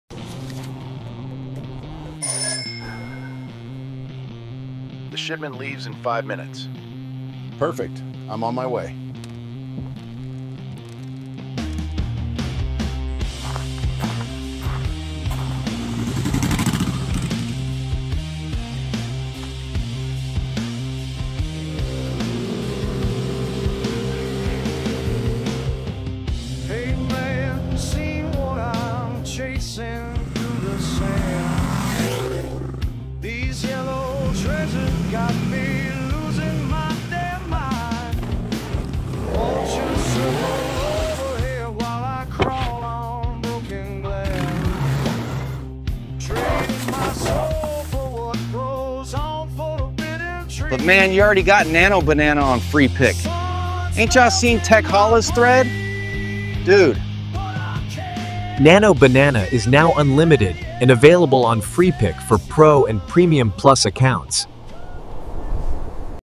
nano-banana ilimitado en Freepik!
El equipo de @freepik  acaba de integrar en su suite nano-banana, el modelo más buscado por todos por su capacidad de editar imágenes o componer nuevas con múltiples elementos.

Y si necesitas un tutorial paso a paso, aquí tienes el que acabo de publicar en X: